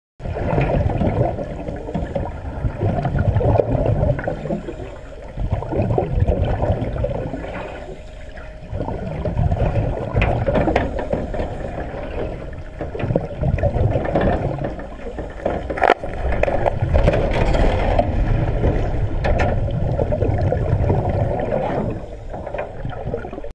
HOW THIS SHARK TRY TO HARM A DIVER
IF YOU LIKE THE CINTENT THEN SUPPORT ME. IN THIS CHANNEL YOU HAVE TO SEE SO MANY TYPES OF CREATURE VIDEO.